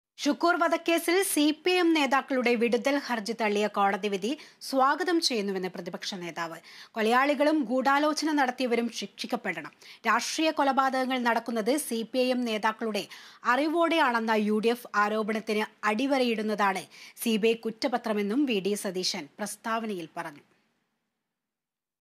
ഷുക്കൂര്‍ വധക്കേസില്‍ കോടതി വിധി സ്വാഗതം ചെയ്യുന്നുവെന്ന് പ്രതിപക്ഷ നേതാവ്
ഷുക്കൂര്‍ വധക്കേസില്‍ സിപിഐഎം നേതാക്കളുടെ വിടുതല്‍ ഹര്‍ജി തള്ളിയ കോടതി വിധി സ്വാഗതം ചെയ്യുന്നുവെന്ന് പ്രതിപക്ഷ നേതാവ്. കൊലയാളികളും ഗൂഡാലോചന നടത്തിയവരും ശിക്ഷിക്കപ്പെടണം. രാഷ്ട്രീയ കൊലപാതകങ്ങള്‍ നടക്കുന്നത് സിപിഎം നേതാക്കളുടെ അറിവോടെയാണെന്ന യുഡിഎഫ് ആരോപണത്തിന് അടിവരയിടുന്നതാണ് സിബിഐ കുറ്റപത്രമെന്നും വി.ഡി സതീശന്‍ പ്രസ്താവനയില്‍ പറഞ്ഞു.

About COA